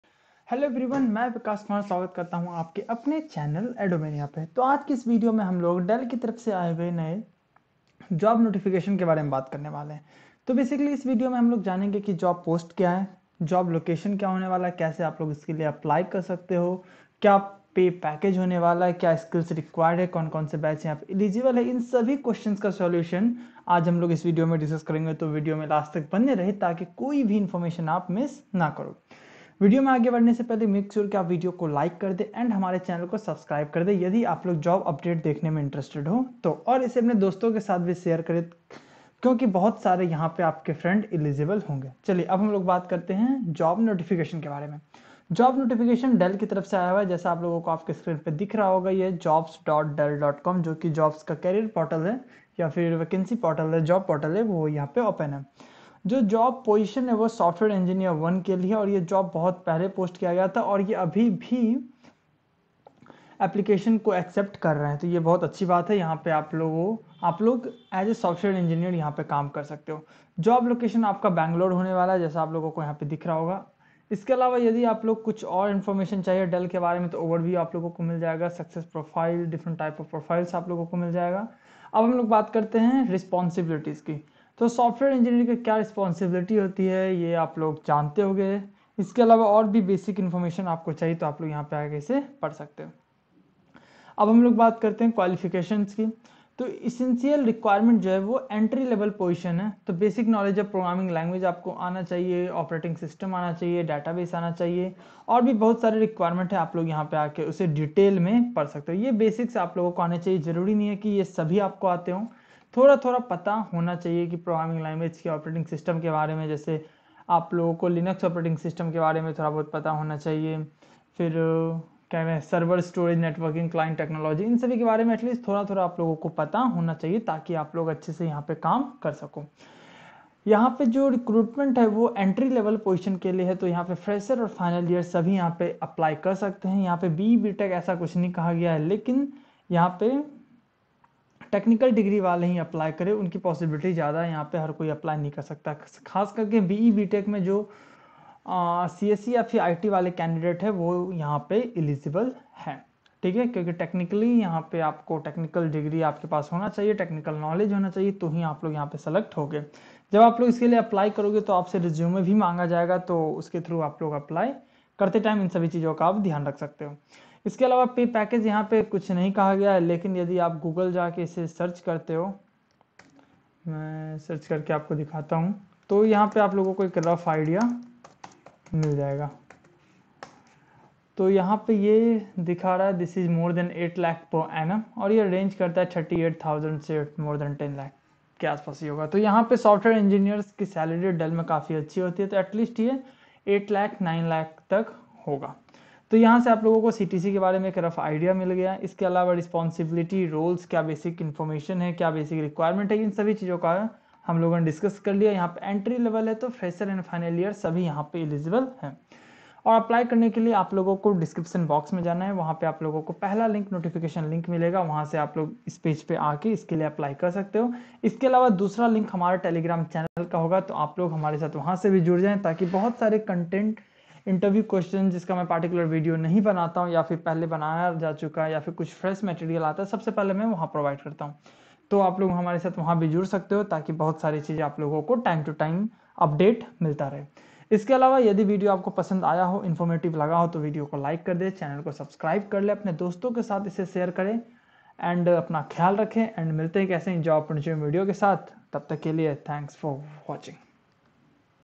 💥Dell Recruitment 2021.|Software Engineer| 0-2| 8+ LPA | B.E/B.Tech| CSE\ECE\IT| Latest Job Update
Dell Off-Campus Recruitment Drive 2021
The latest Job Update for all Fresher and Final Year Candidates have been released by Dell.
Dell is offering for Software Engineer Role.
Dell Hiring Off-Campus Hiring 2021 for all the candidates who are residents of India.
All the candidates who are looking for jobs and having a dream to be part of a well-established recognize company are getting a chance to fulfill their Dream.
You are just one step away to make your dream true.
Dell Hiring Off-Campus Hiring  2021 is Active Now.
All Interested Candidates Apply on the Given Below Link as soon as possible.
It is a golden opportunity for all the candidates who have recently graduated with any discipline.
Jobs for Fresher Candidates.
✌✌✌If You like my work and affords,,,, PLEASE

📢📢JOIN OUR TELEGRAM CHANNEL FOR LATEST UPDATES📢📢

🙄🙄ANY QUERY/CONFUSION🙄🙄